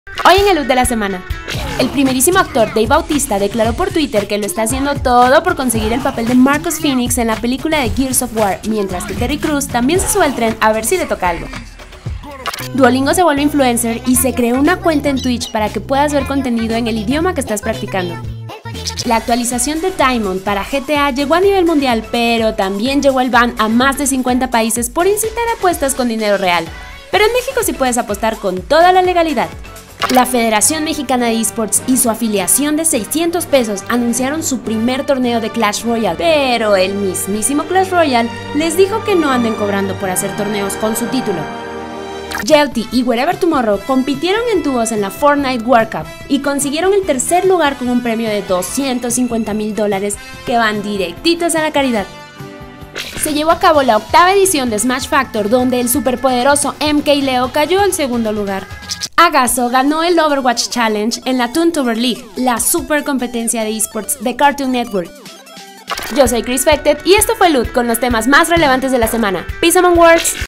ElLoot y Dave Bautista en Gears of War
Twitch quiere aprender más idiomas, Werevertumorrow aprendió de un pro de fortnite, Cartoon Network aprende de esports, la FEMES aún no aprende... Y más en ElLoot🪂 de la semana.